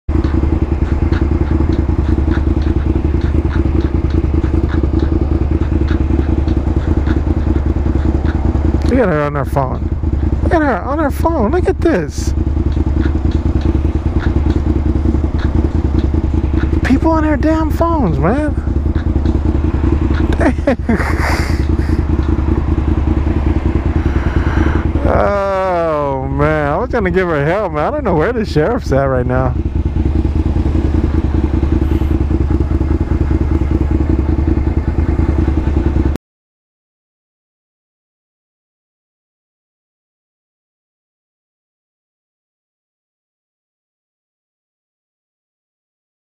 Distracted Driver|DWOP| Lady in Lexus chats on phone while driving| San Marcos CA
Driving and talking on the phone in your hand is illegal in most states(California Vehicle Code 23123.5). Driving while on the phone (DWOP). While waiting at a light I see this Lady in Lexus pull up in the right turning lane with her phone in her hand talking on it .Come on people with the advance technology in wireless communication and Bluetooth there is no excuse for this now. 😟😟😟😟